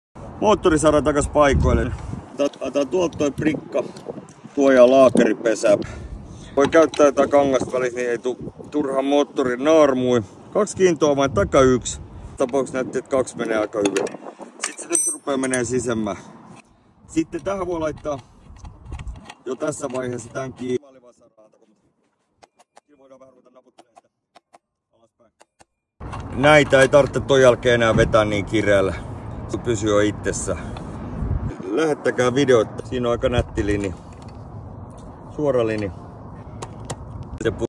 Kesä 2024 Meeko Pro moottoripyörä eli takapyörä, miten saat sen hetkessä takaisin paikoilleen.
Meekon moottori on joskus tiukka saada takaisin. Koita tätä ideaa ja varmasti säästät aikaa ja vaivaa. Tarvitset kaksi kiintoavainta, katso video.